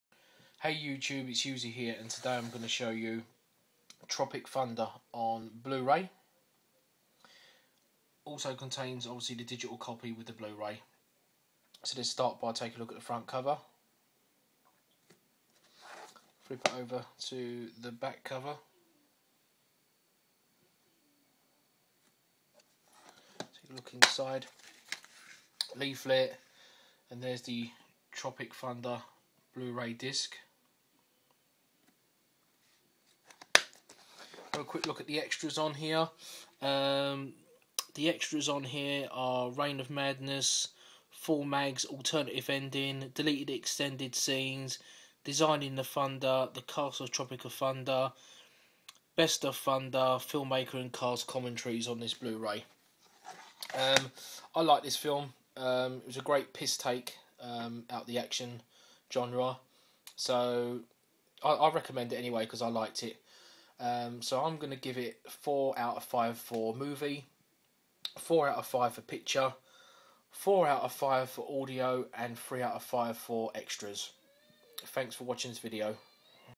Tropic thunder blu-ray review
Showing tropic thunder blu-ray

Movie 4/5
Video 4/5
Audio 4/5
Extars 3/5